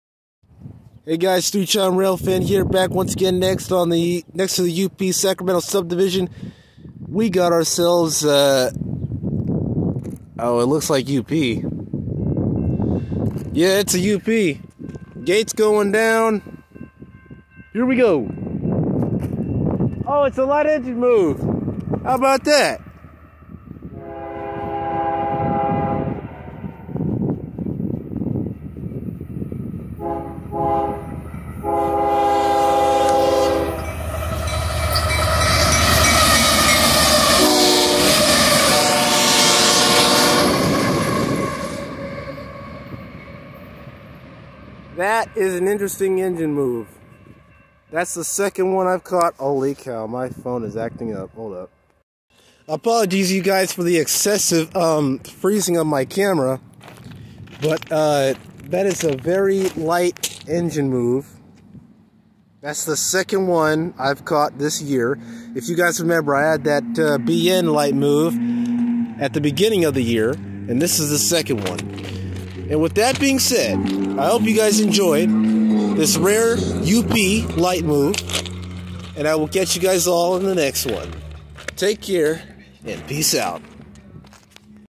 Rare UP Light Move!
Just like at the beginning of the year when I caught a rare BN light move, it was then UP's turn to run 3, north up the Sac Sub to work at the South Sac yard, as part of a work train. The crew ere friendly and gave us a toot toot on the way by. Hope you guys enjoy!

Locomotives:
UP GE AC45CCTE 5529 (leading)
UP GE AC44CWCTE 5923 (trailing)
UP GE AC44CWCTE 5868 (trailing)

Location: Bilby Rd in Elk Grove, CA